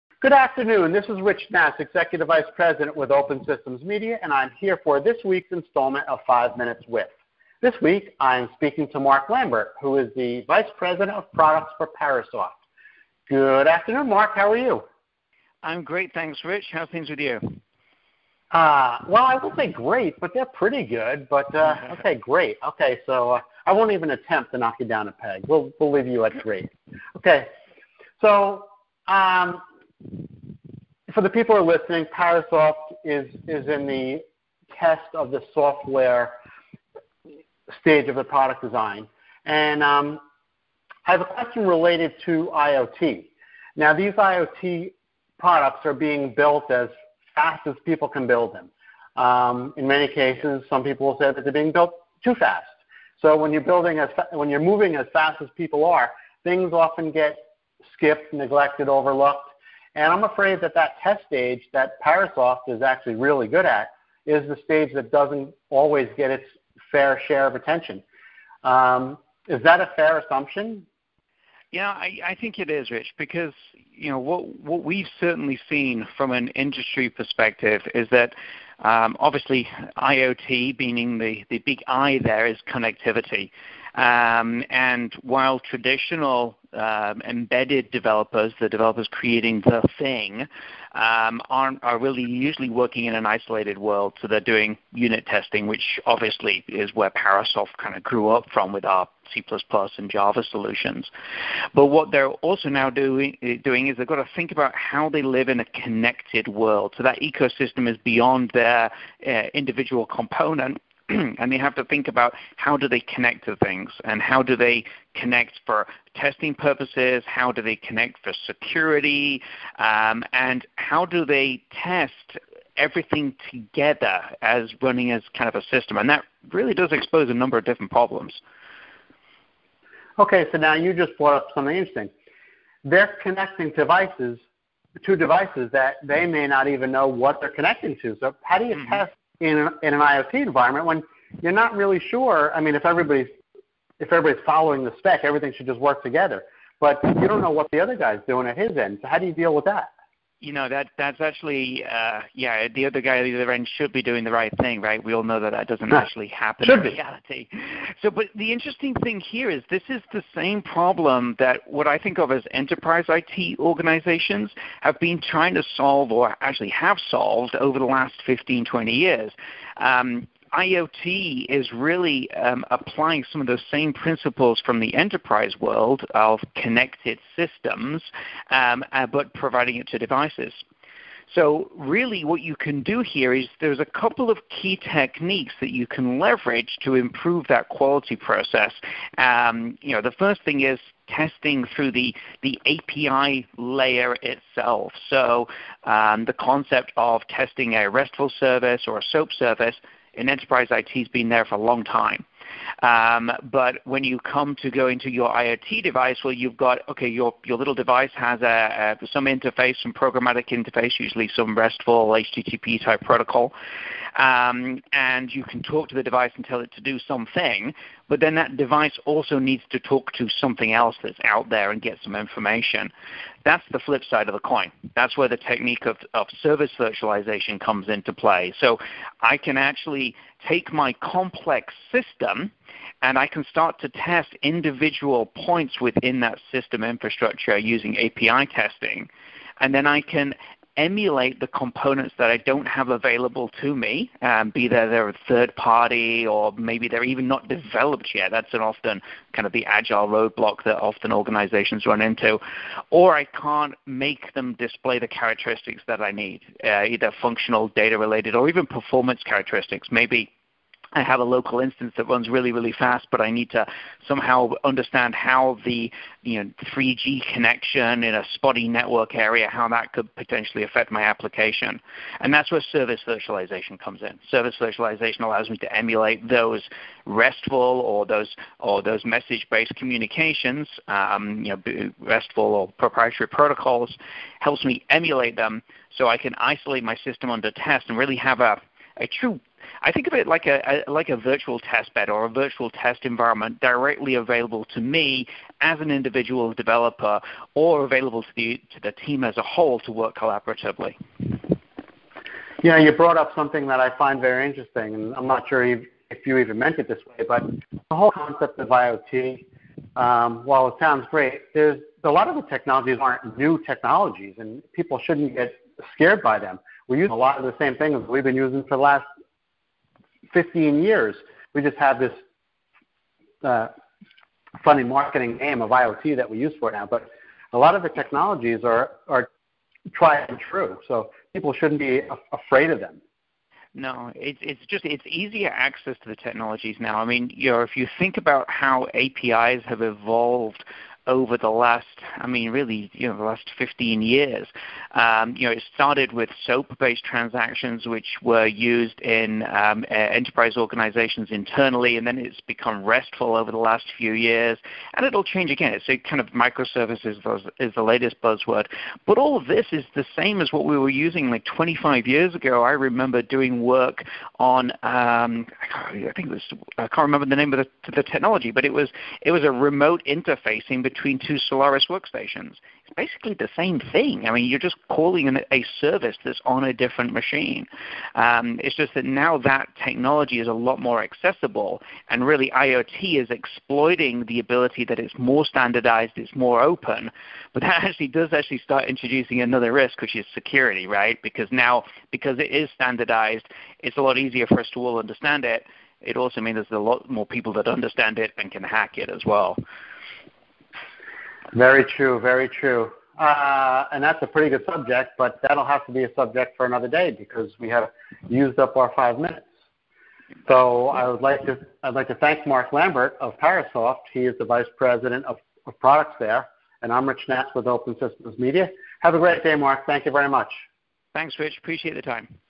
Five Minutes With…Mark Lambert, VP, Parasoft
IoT design are flying out the door at a rate that could be termed as scary. The pressure on the engineering team to meet the current design windows is enormous. In this race to be first, something’s gotta give. Unfortunately, it’s the test phase that often gets the short shrift. What do we do about that? That’s the question I posed to Mark Lambert, Parasoft’s Vice President of Products. His response revealed a domino effect of issues, most of which you need to be aware of.